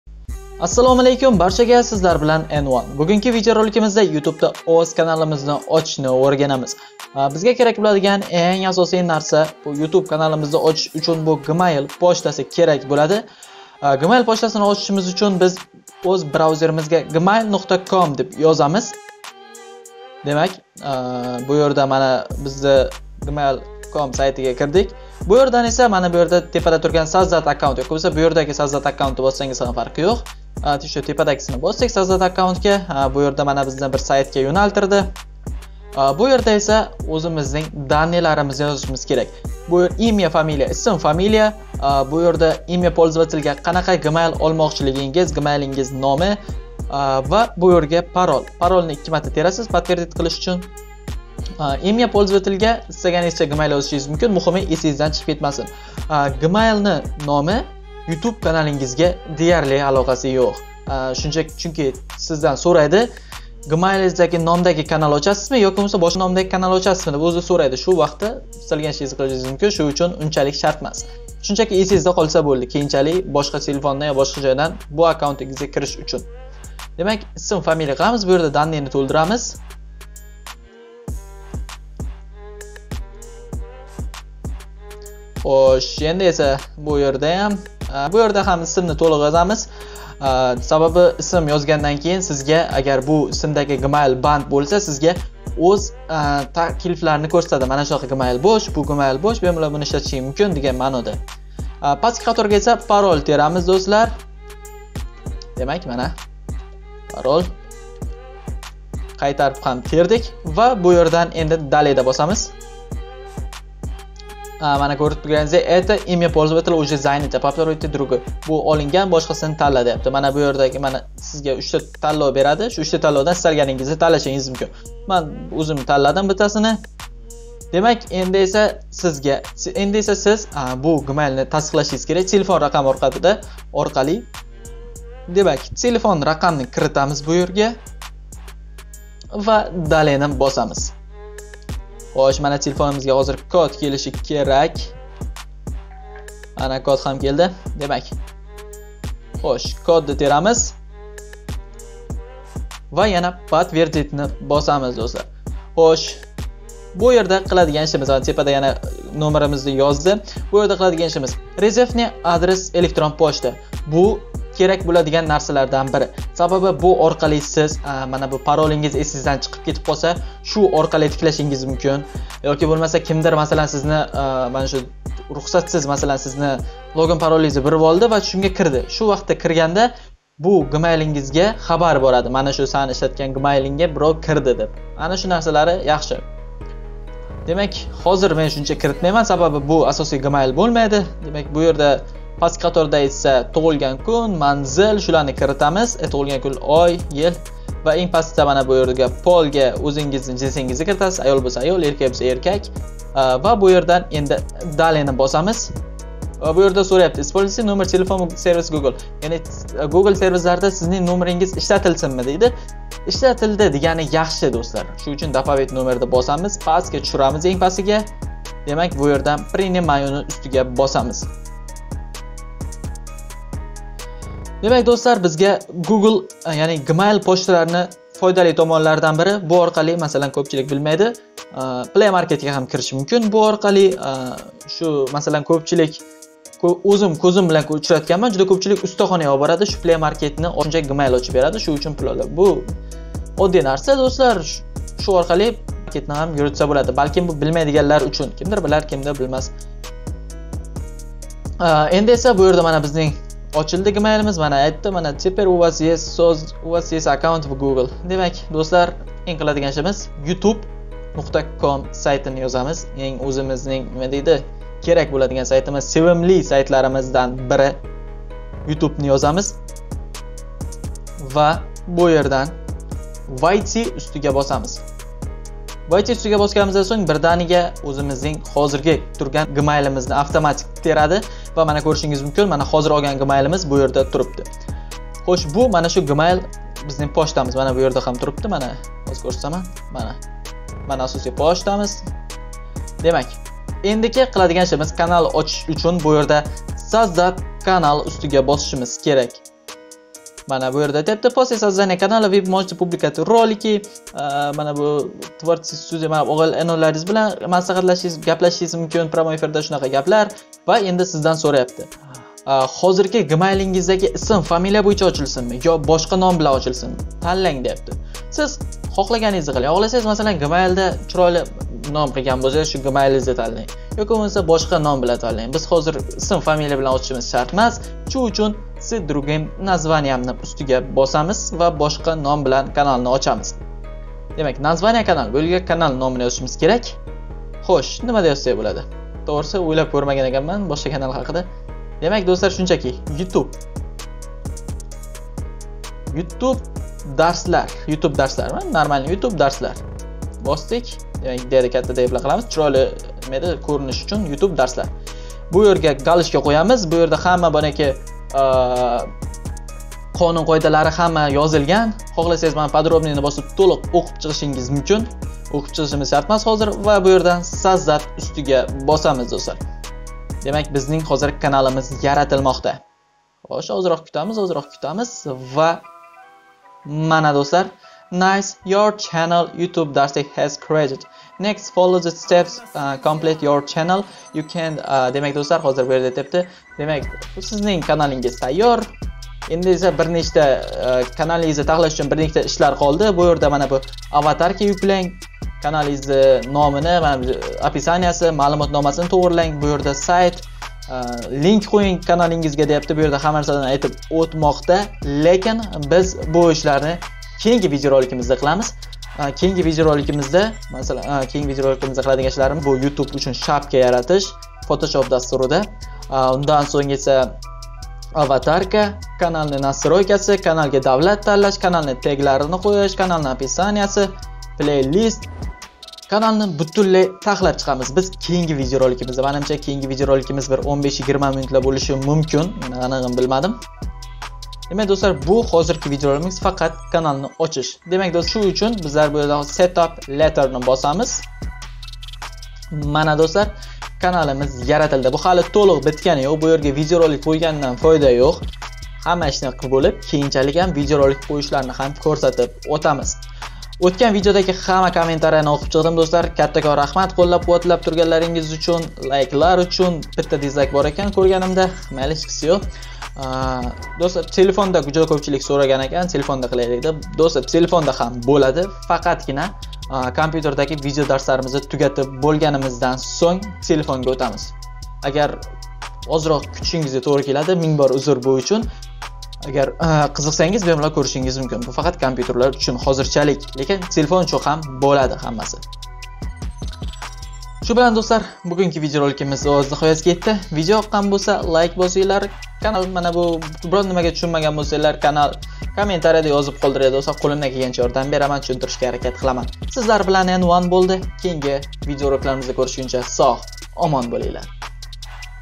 YOUTUBE DA KANAL OCHISH / YOUTUBE VIDEO DARSLAR 0 DAN
YOUTUBEDA KANAL OCHISH / YOUTUBE VIDEO DARSLAR 0 DAN

ASSALOMU ALAYKUM SIZNI NONE KANALIDA KO'RIB TURGANIMDAN JUDA HAM XURSANDMAN. USHBU VIDEO DARSLARIMIZDA SIZGA YOUTUBEDA KANAL OCHISHDAN BOSHLAB KANALGA VIDEO YUKLAB VIDEO USTIGA RASM QUYISHGACHA O'RGATIB BORISHGA HARAKAT QILAMIZ
VIDEO SIZGA YOQDIMI UNDA VIDEOGA LIKE BOSIB KANALGA OBUNA BO'LISHNI UNUTMANG.